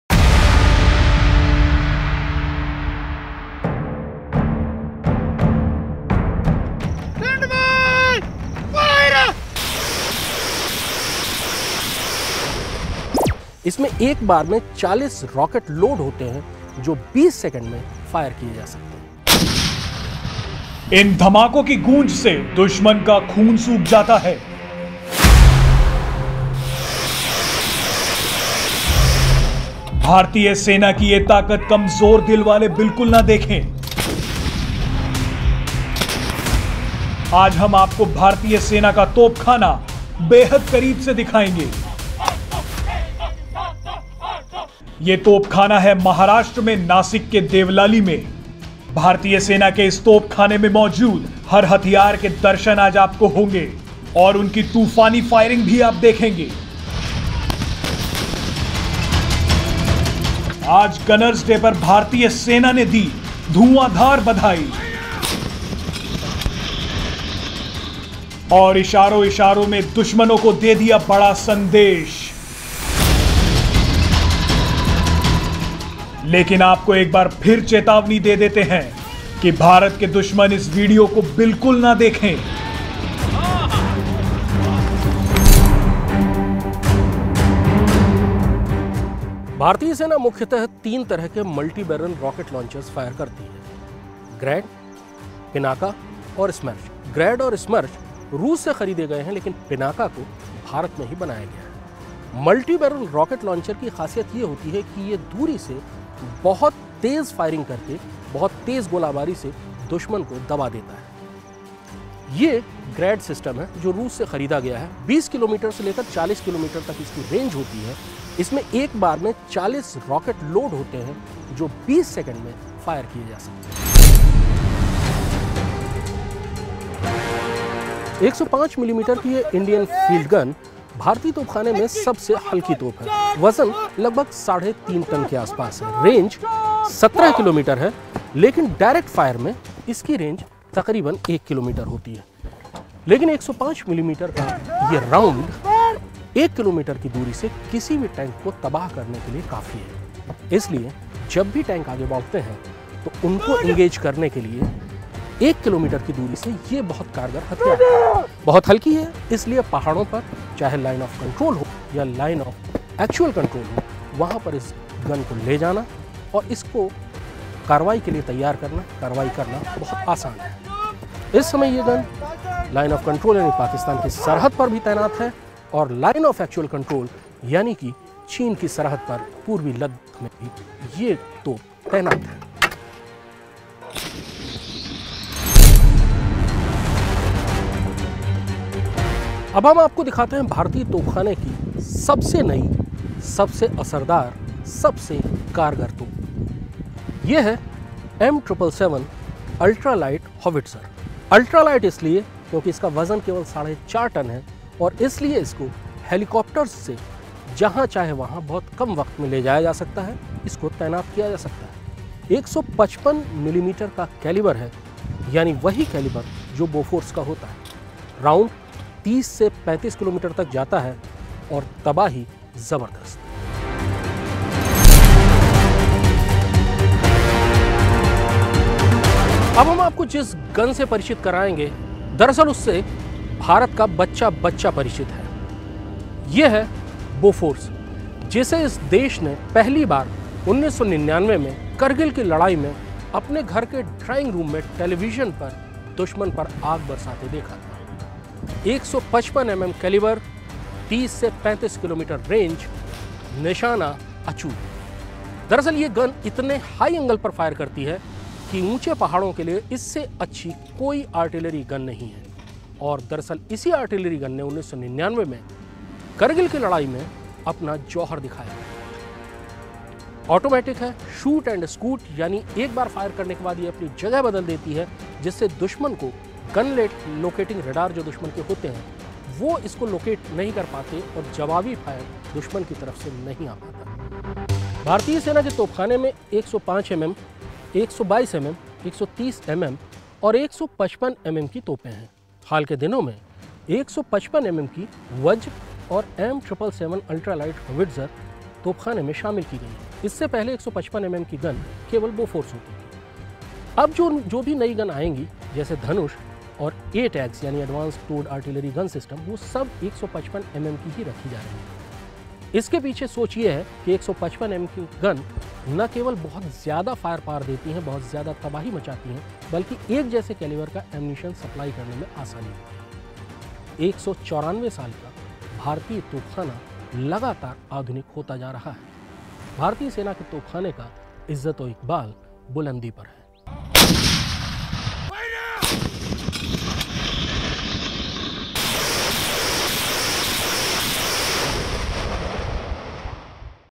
भारतीय सेना की ये ताकत कमजोर दिल वाले कतई ना देखें ! | Indian Army | Gunners Day
आज हम आपको भारतीय सेना का तोपखाना बेहद करीब से दिखाएंगे, ये तोपखाना है महाराष्ट्र में नासिक के देवलाली में.. भारतीय सेना के इस तोपखाने में मौजूद हर हथियार के दर्शन आज आपको होंगे और उनकी तूफ़ानी फ़ायरिंग भी आप देखेंगे..